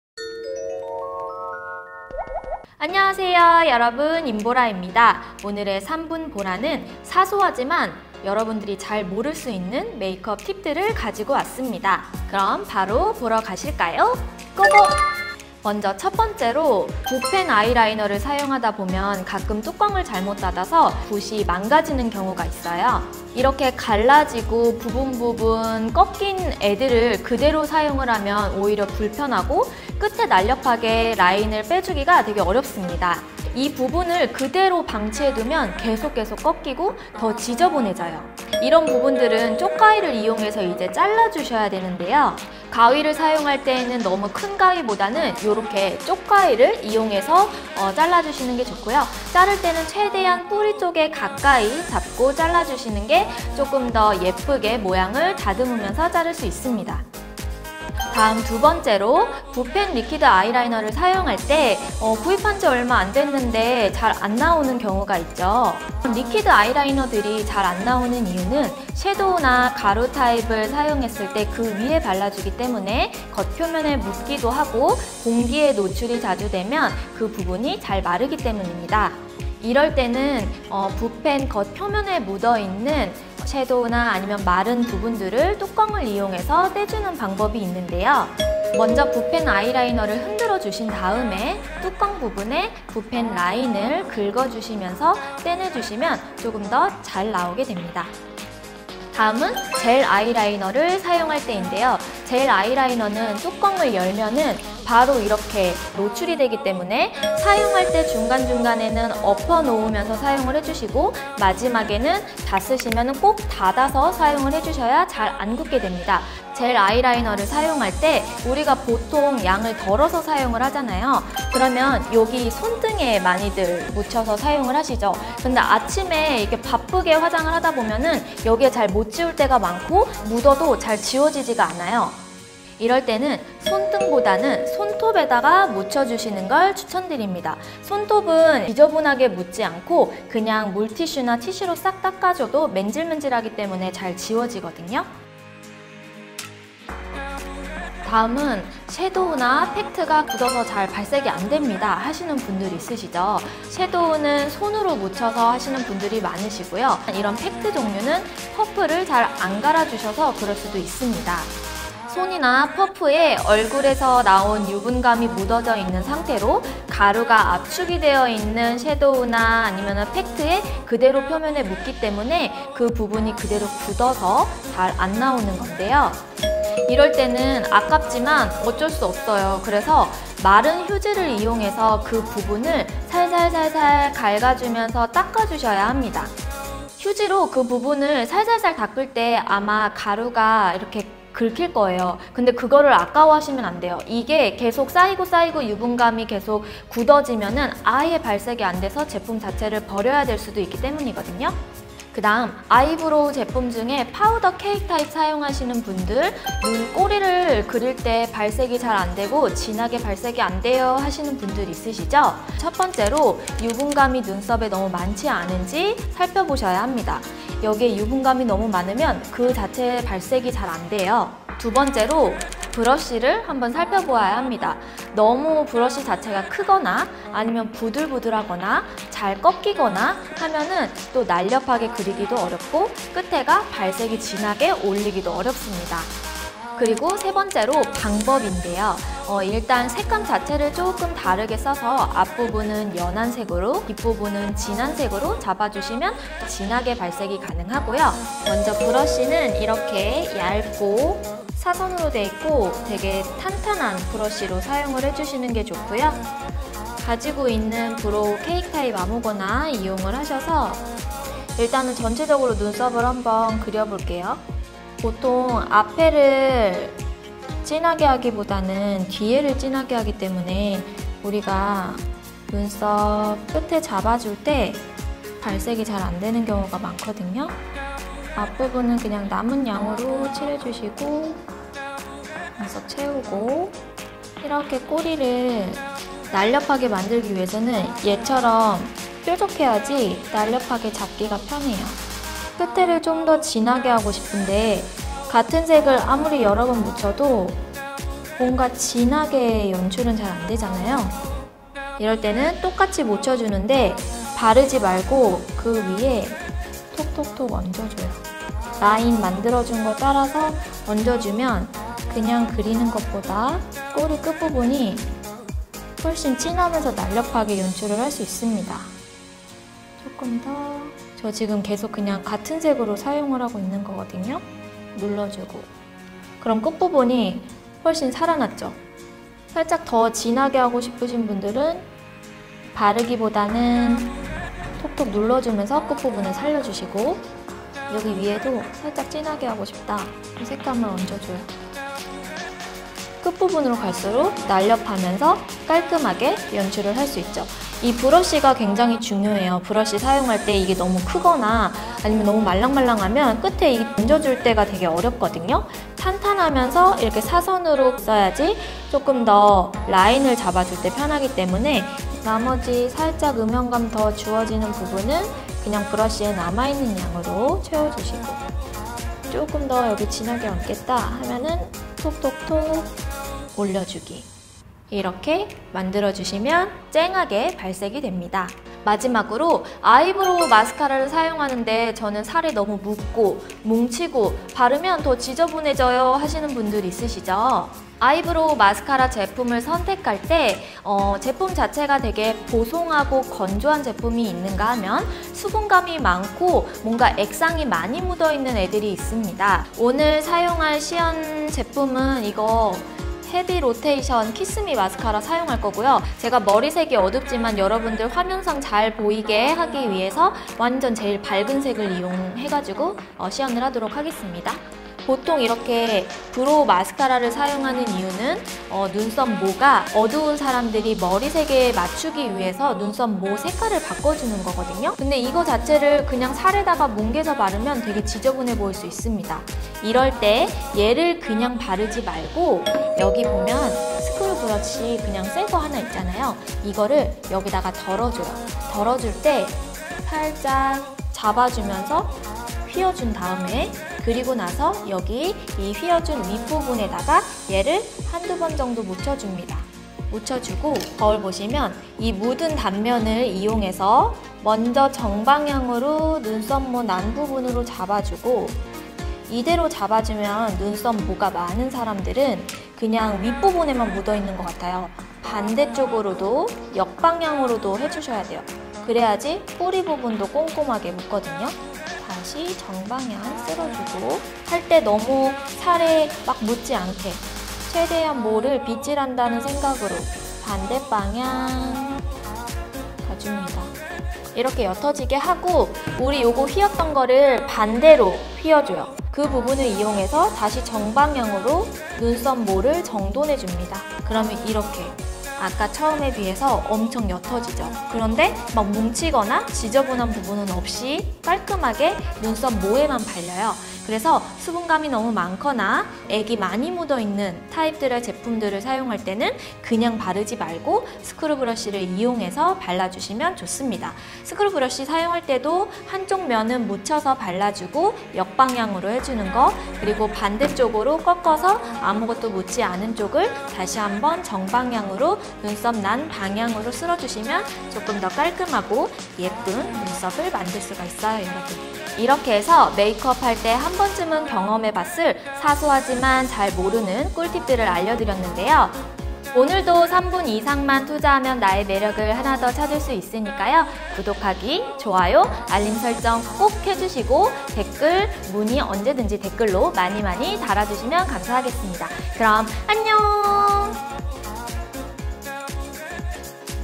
[삼분보라] 메이크업한다면 꼭 필요한 7가지 꾸르팁! (feat.아이라인,섀도우,브로우마스카라관련!)
안녕하세요 인보라입니다!
오늘의 삼분보라는! 메이크업하다보면!! 꼭 필요한! 사소한 꿀꿀꿀팁!!을
영상으로 가져왔어용!! :)

인스타 in_bora_

사용제품
*브로우브러쉬
*브로우마스카라